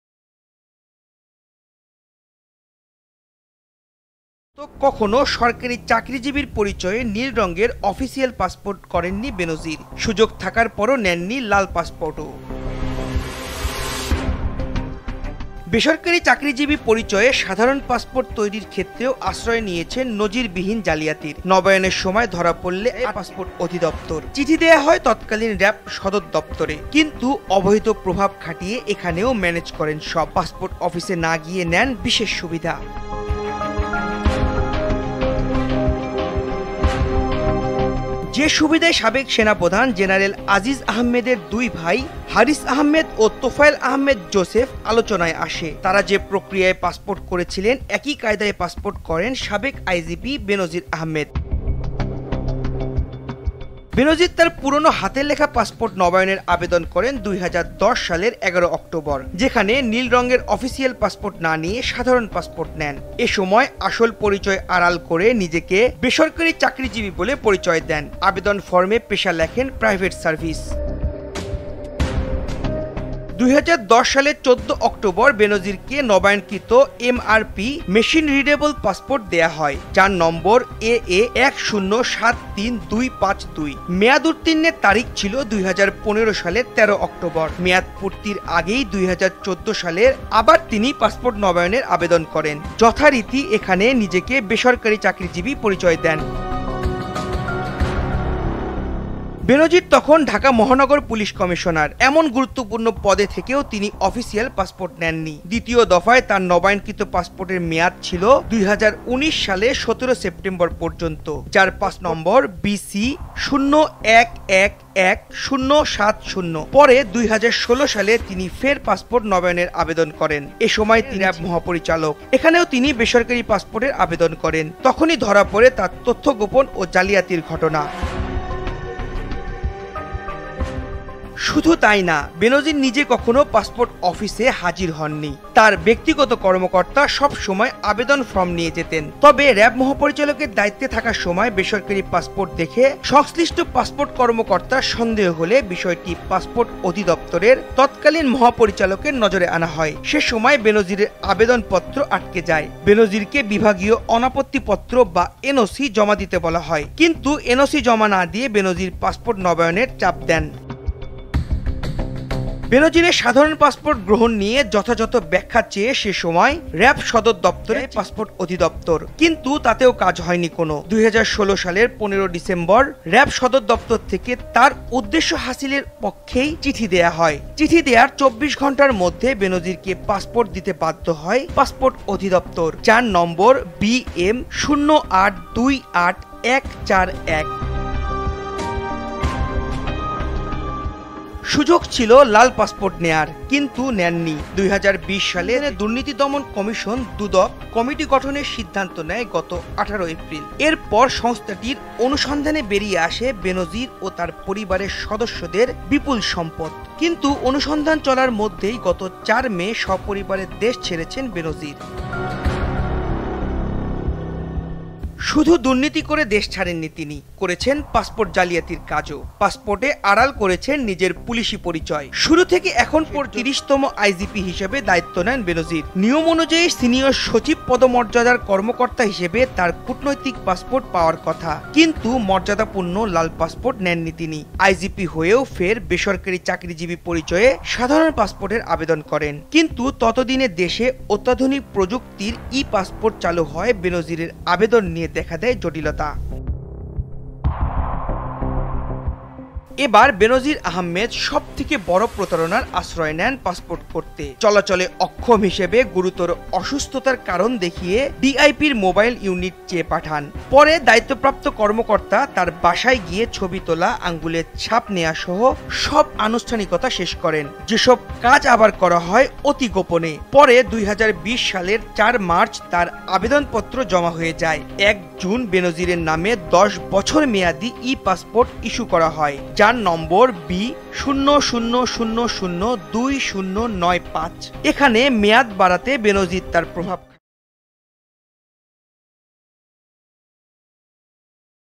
পাসপোর্টেও নজিরবিহীন জালিয়াতি বেনজীরের#facebook
বেনজীর আহমেদ নিজের পাসপোর্ট করতেও করেছেন নজিরবিহীন জালিয়াতি। পুলিশ পরিচয় আড়াল করে পাসপোর্টে নিজেকে ‘বেসরকারি চাকরিজীবী’ পরিচয় দিয়ে নিয়েছেন সাধারণ পাসপোর্ট। সরকারি চাকরিজীবী পরিচয়ে নীল রঙের অফিশিয়াল পাসপোর্ট করেননি তিনি। সুযোগ থাকার পরও নেননি ‘লাল পাসপোর্ট’।

সাবেক সেনাপ্রধান জেনারেল আজিজ আহমেদের আলোচিত দুই ভাই হারিস আহমেদ ও তোফায়েল আহমেদ জোসেফ যে প্রক্রিয়ায় পাসপোর্ট করেছিলেন, ঠিক সেই একই কায়দায় পাসপোর্ট নেন বেনজীর। চাঞ্চল্যকর এসব তথ্য একটি গণমাধ্যমের অনুসন্ধানে জানা গেছে।

জানা যায়, বেনজীর তার পুরনো হাতে লেখা পাসপোর্ট নবায়নের আবেদন করেন ২০১০ সালের ১১ অক্টোবর। রহস্যজনক কারণে নীল রঙের অফিশিয়াল পাসপোর্ট না নিয়ে সাধারণ পাসপোর্ট নেন তিনি। এ সময় আসল পরিচয় আড়াল করে নিজেকে বেসরকারি চাকরিজীবী বলে পরিচয় দেন। আবেদন ফরমে পেশা হিসাবে লেখেন ‘প্রাইভেট সার্ভিস’।